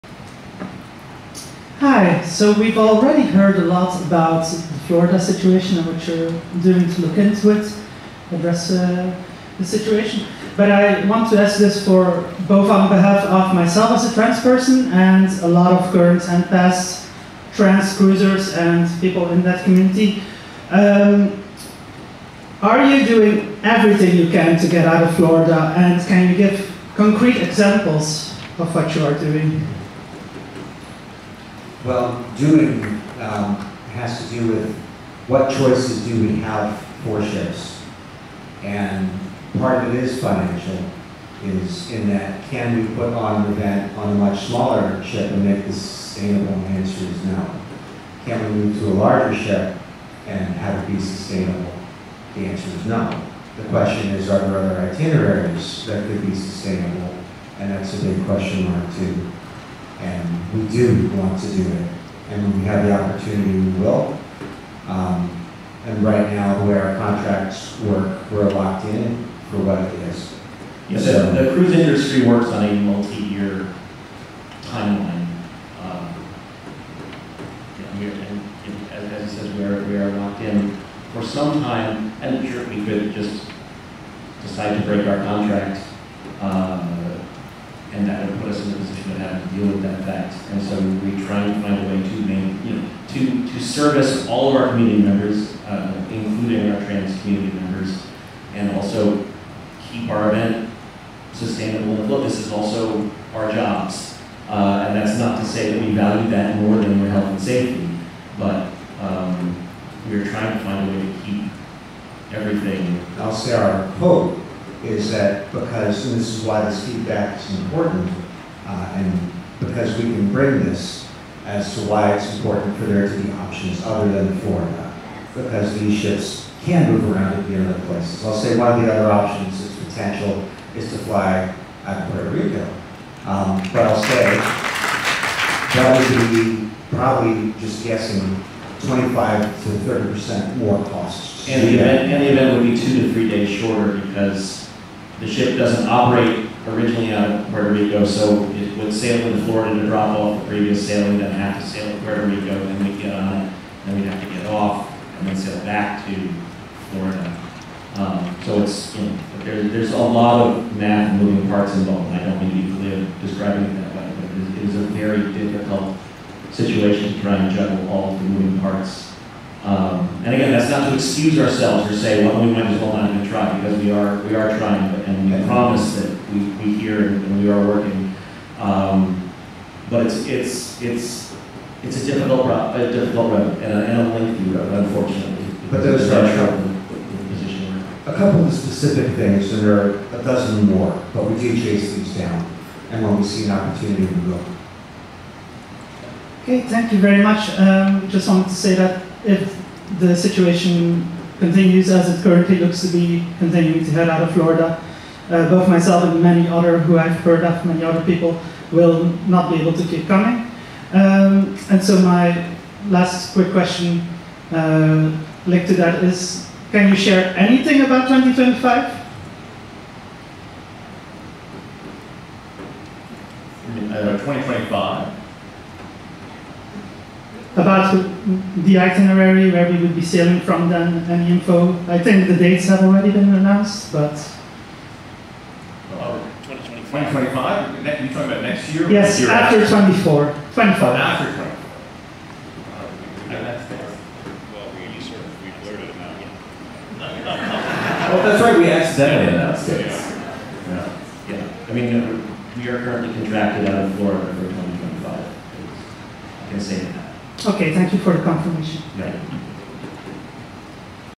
Are you doing everything you can to get out of Florida? What about 2025? — JoCo Cruise 2023 Q&A
Q: Are you doing everything you can to get out of Florida, and can you give concrete examples? Can you share anything about 2025?

A: (I did not take notes on the concrete examples) Unfortunately they are contracted to leave out of Florida in 2025.

All the Q&A I've recorded from JoCo Cruise and JoCo Cruise-adjacent performers and performances can be found